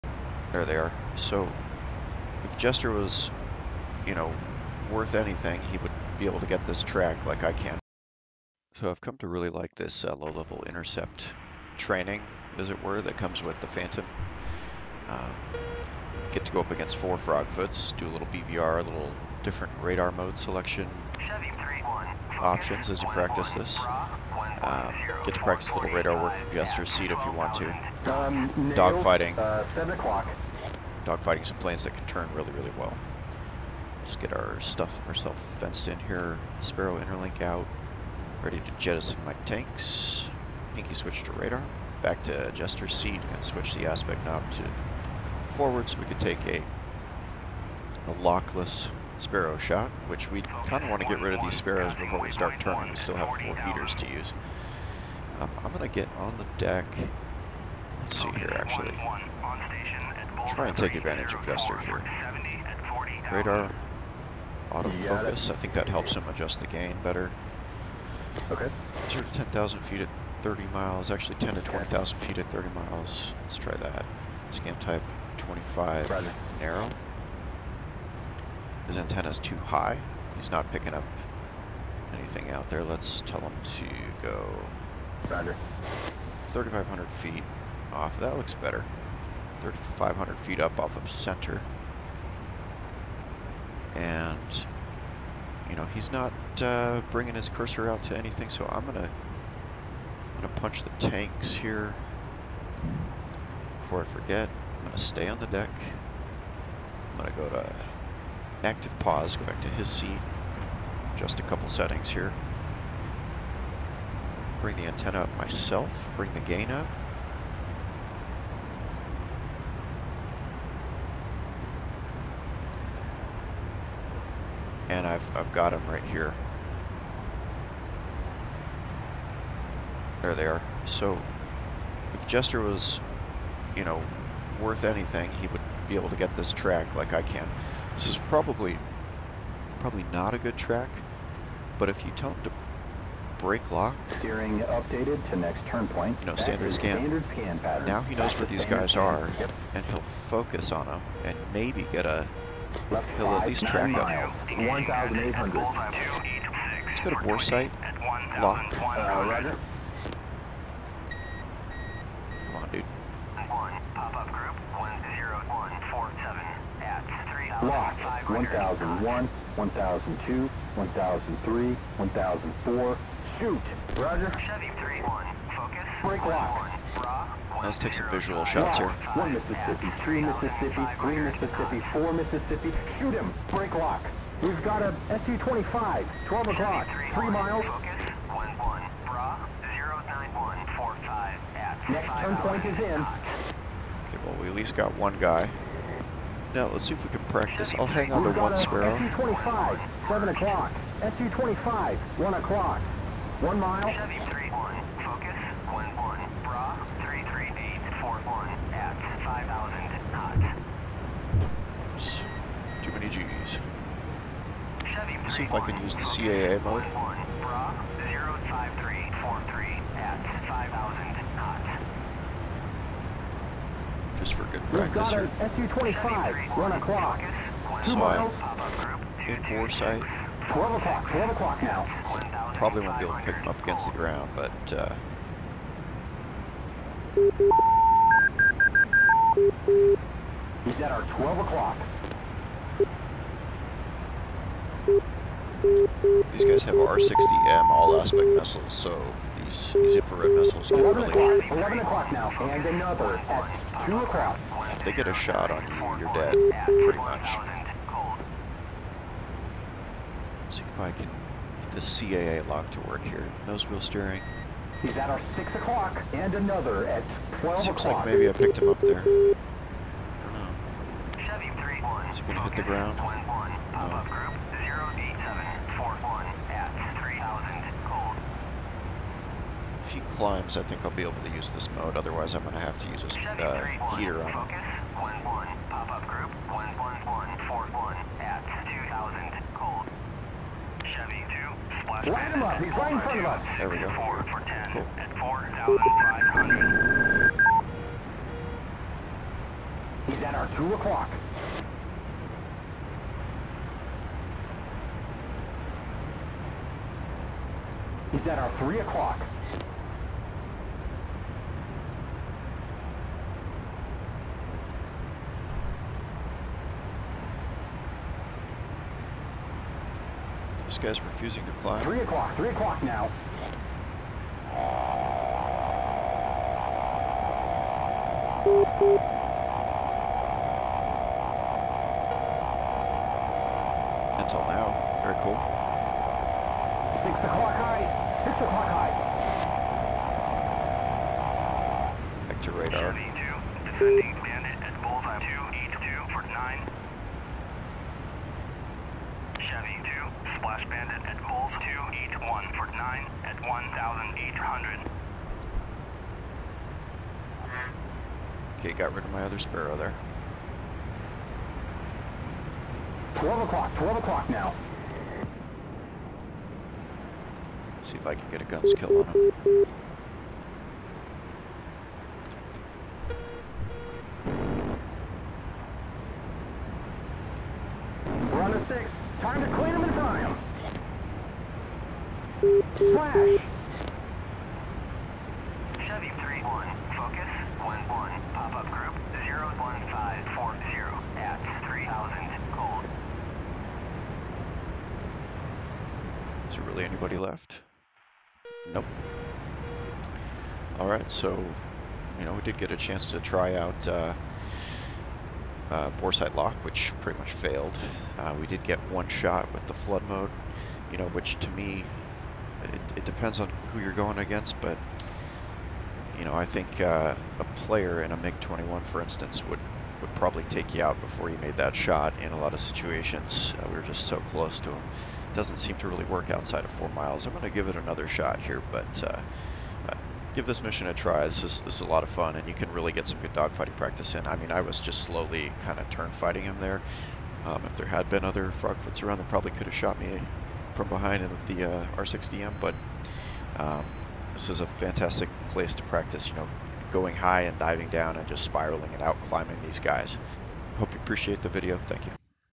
F4 Phantom Intercepts 4 Frogfoots low level- Jester can't hack it?
Thinking BVR / WVR strategy before the merge, using the WSO seat radar to do what Jester can't (quickly point where AWACS GCI is telling you to), trying out some different AIM-7 sparrow employment methods - boresight (5 nm via CAGE mode- fails), CAA (fails), "flood" (success!)